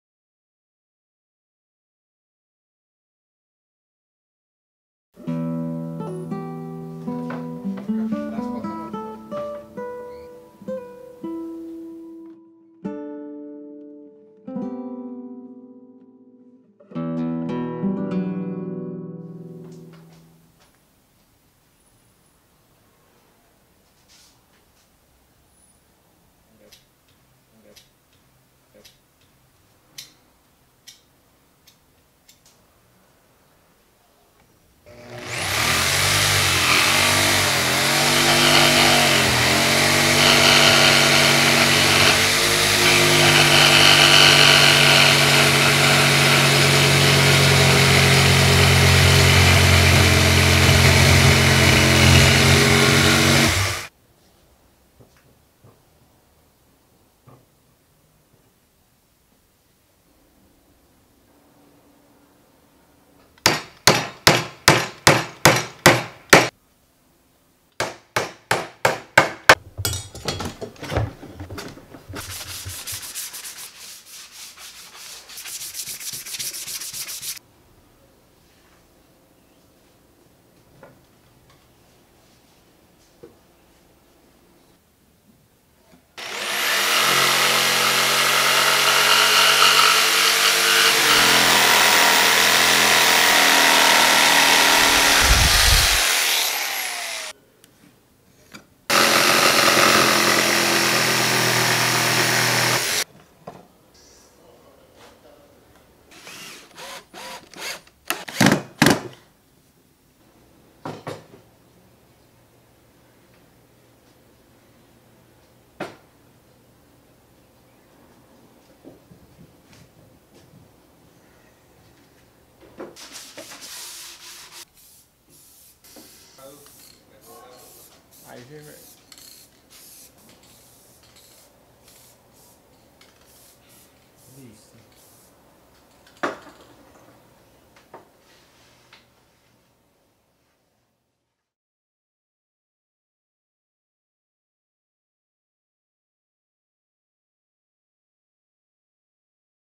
Amaro [Midioslepague]
Amaro, cotidiano.

Corta, mide, traza. Sonríe.

Un retrato a un personaje que vive entre retazos, que vive en el afán. Y en el afán vive con calma.